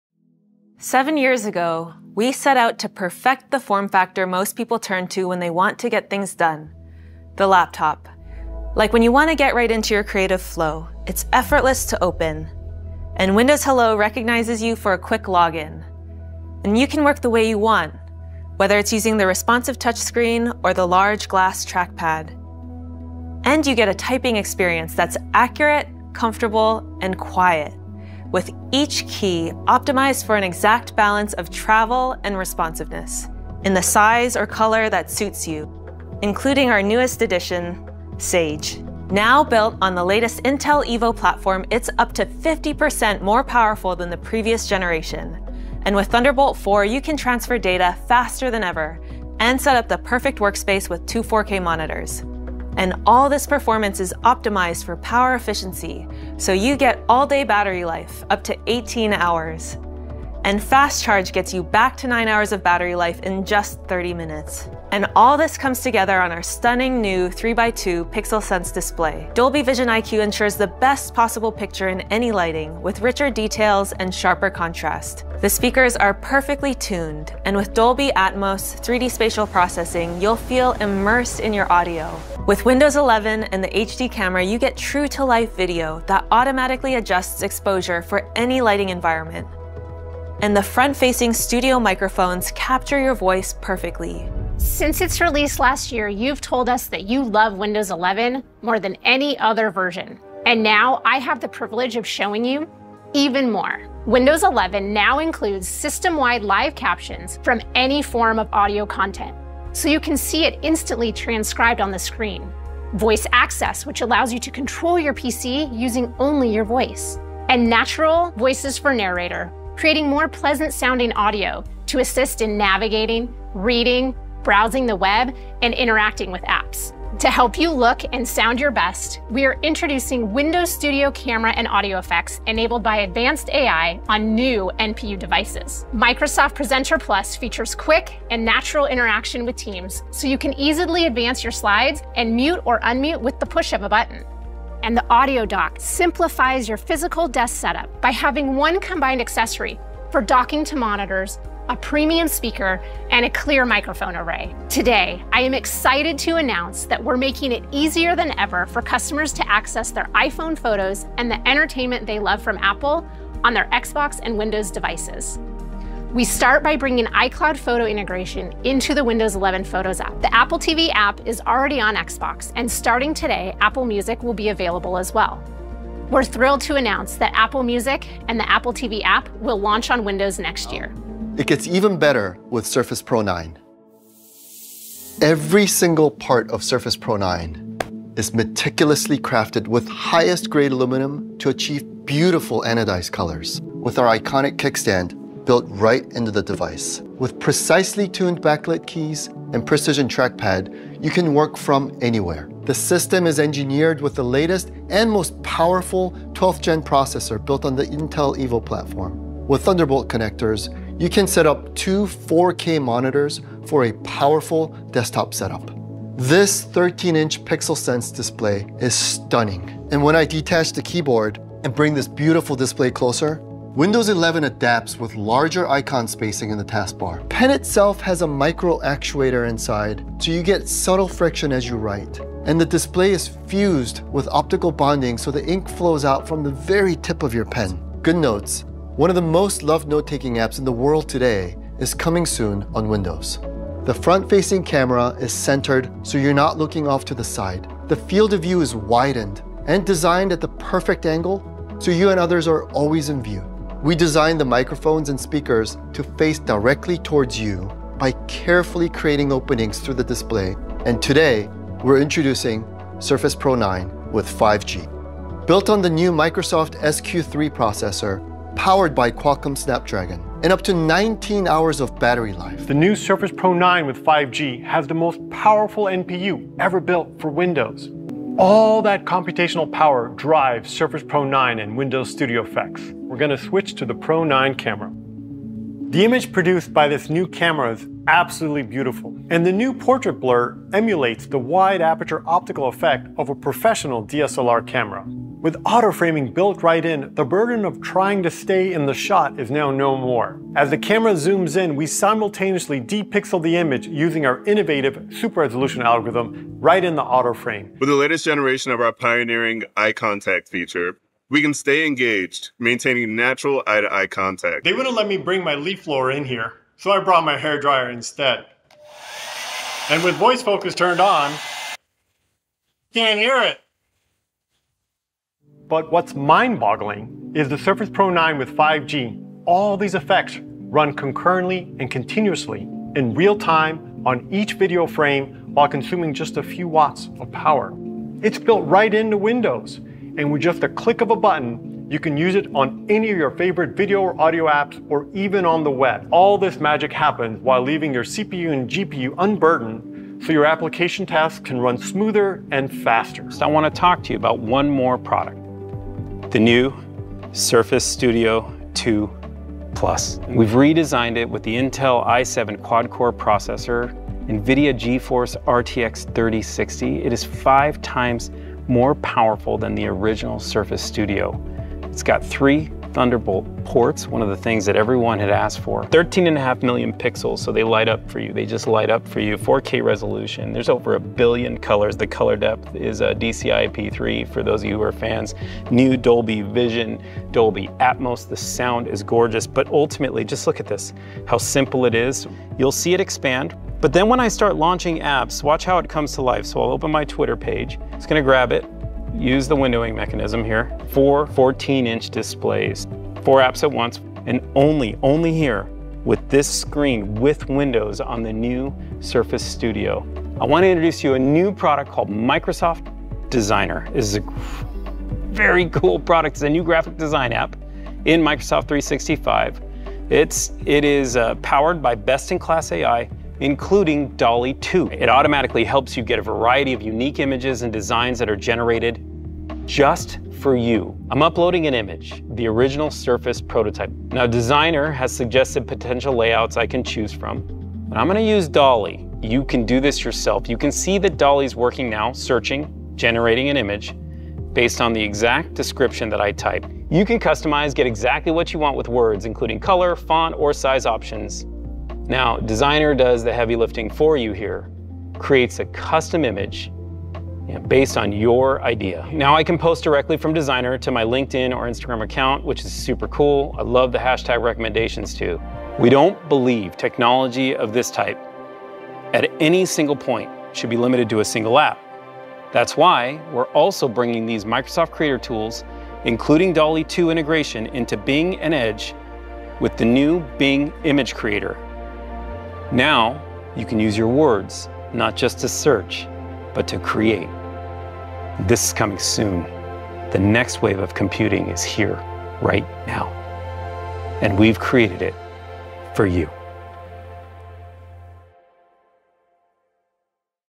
Microsoft Surface 2022 event under 8 minutes
Microsoft has launced its 2022 Surface at Surface event, where it showed off its new Surface Laptop 5, Surface Pro 9, and Surface Studio 2 Plus with new accessories and software, including a Designer app powered by Dall-E 2.

0:00 Surface Laptop 5
1:34 Windows 11 Updates
2:17 Microsoft Presenter+
2:25 Microsoft Audio Dock
2:35 Integration with Apple
3:02 Surface Pro 9
5:53 Surface Studio 2 Plus
6:53 Microsoft Designer with Dall-E 2